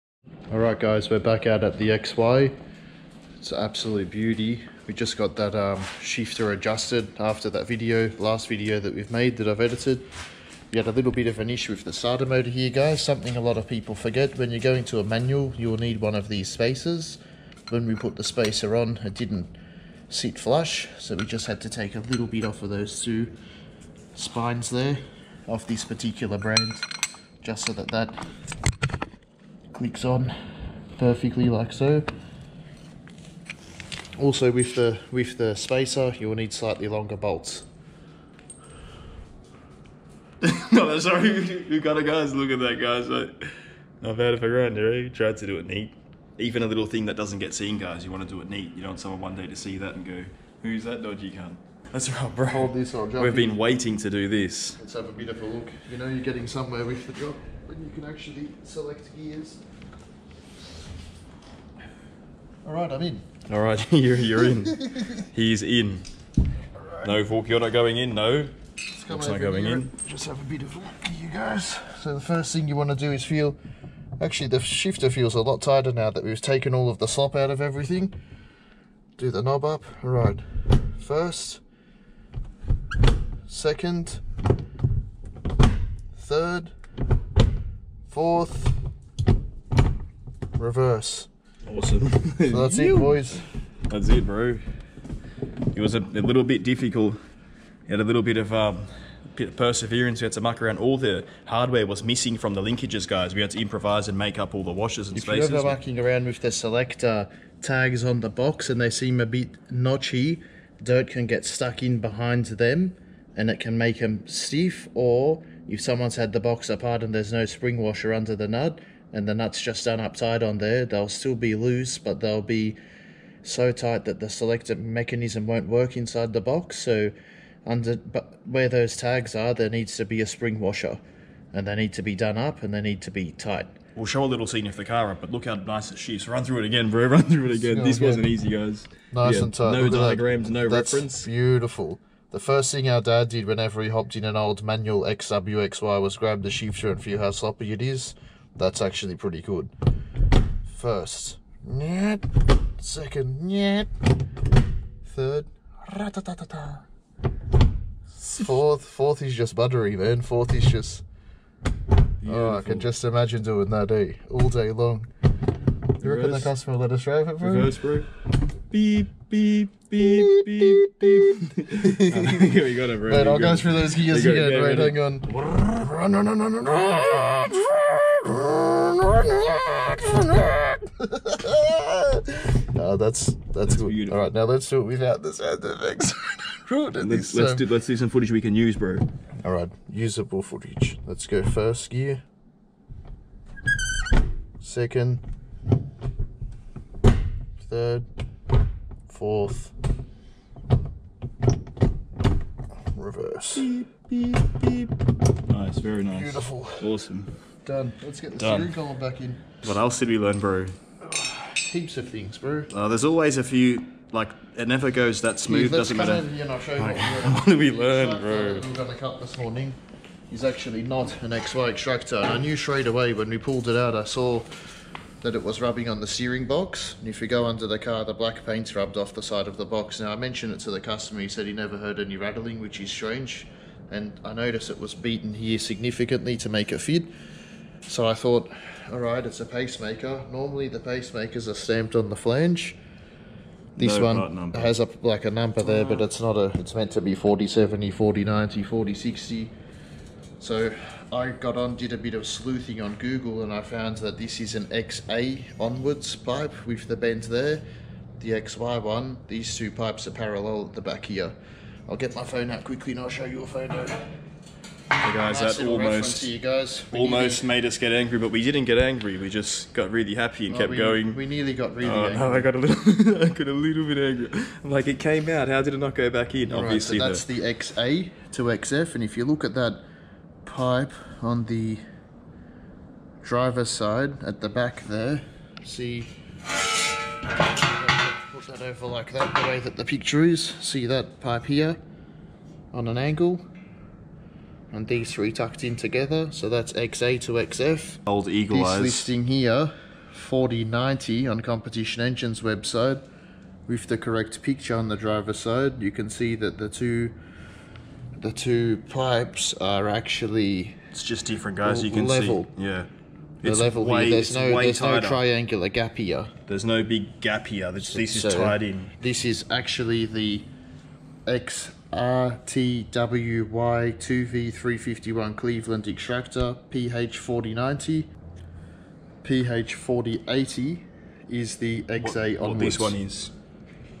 DIFFERENCE BETWEEN XY AND XA 351 CLEVELAND EXTRACTORS
G'day guys, In this video I explain the difference between XR XT XW XY and the XA XB XC XD XE Pacemaker Extractors. This should help a lot of people who are looking for secondhand ones to ensure they buy the correct set.